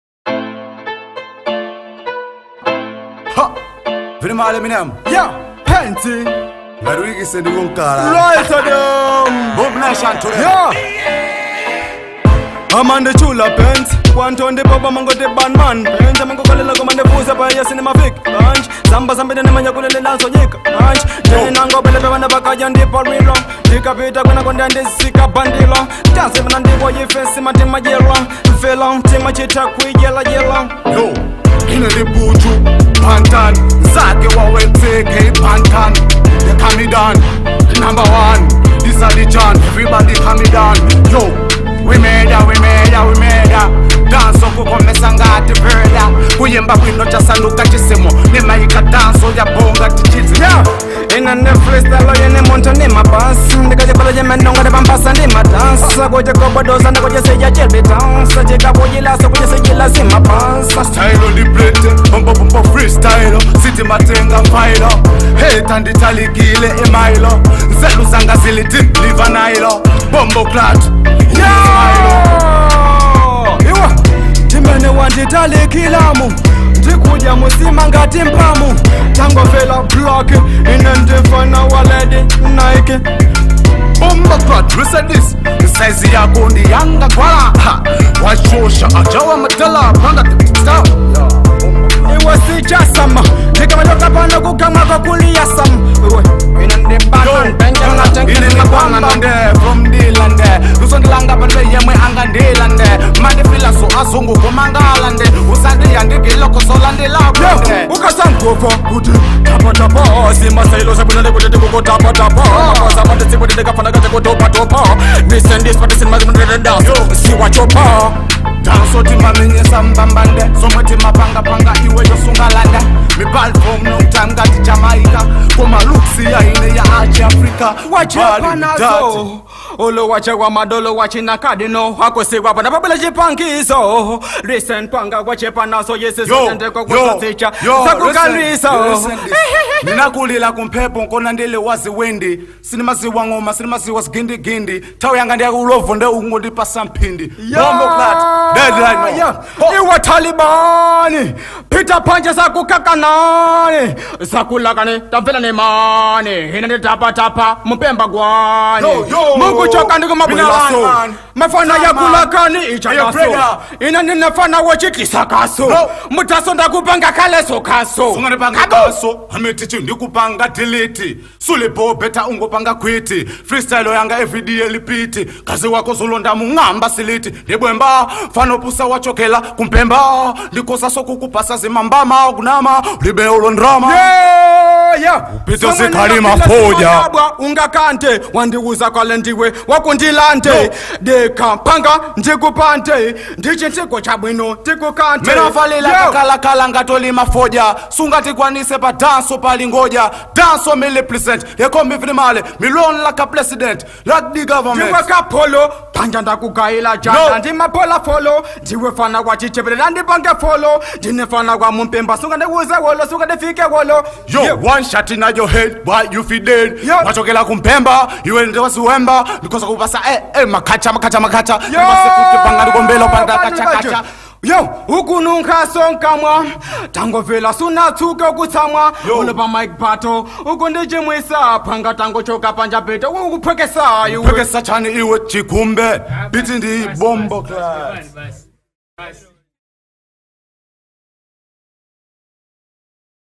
MIC BATTLE COMPETITION - PNG(Red T-shirt) Vs Vin Marley (Black T-shirt) Who Do You Vote For?
Round 1 CONTINUES